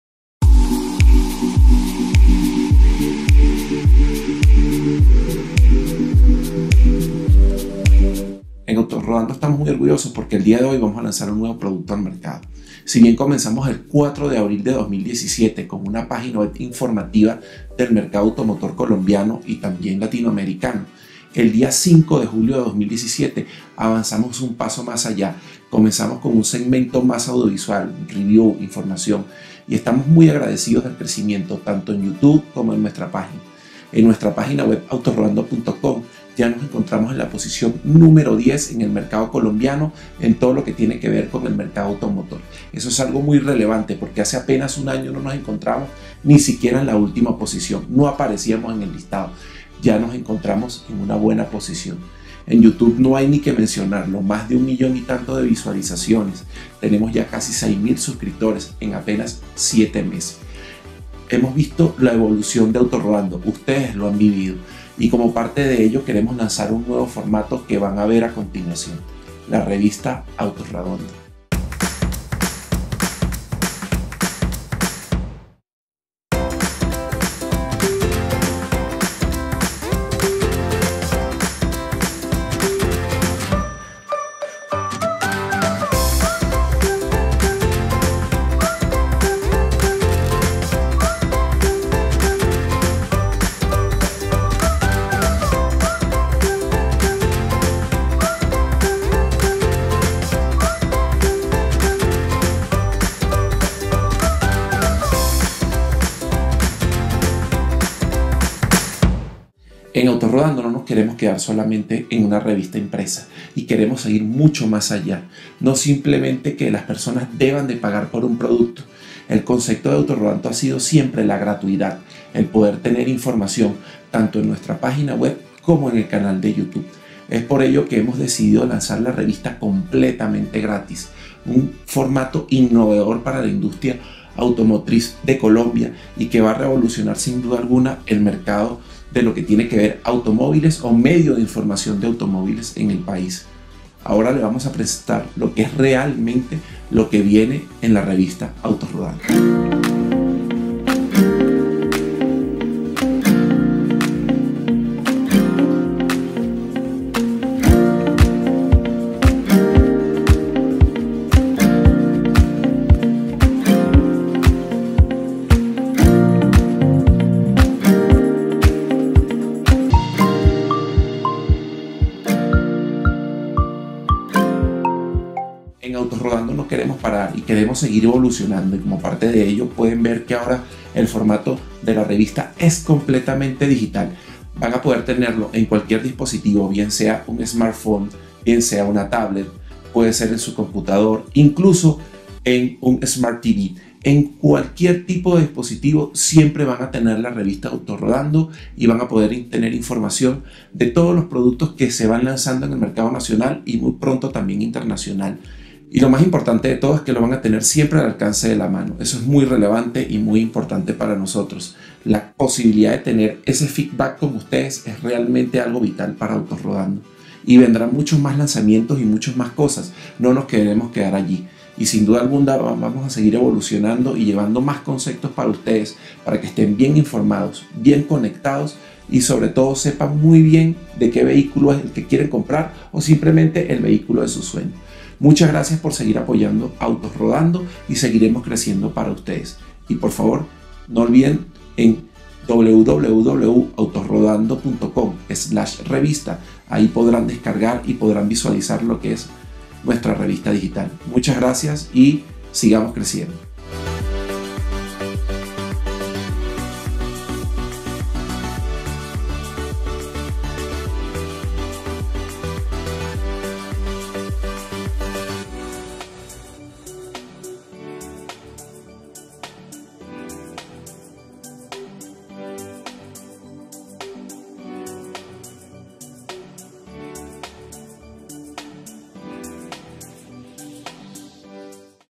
Lanzamiento Revista Autos Rodando
Estamos orgullosos de presentarles la Revista Autos Rodando, la cual podrás ver desde cualquier dispositivo sea SmartPhone, Tablet, computador o SmartTV, no olvides visitar la Revista Autos Rodando Digital

Te mostramos los últimos vehículos en el mercado Colombiano y Latinoamericano, y del Mundo desde los más costosos, hasta los más económicos. Todas las semanas tendrás un vídeo nuevo.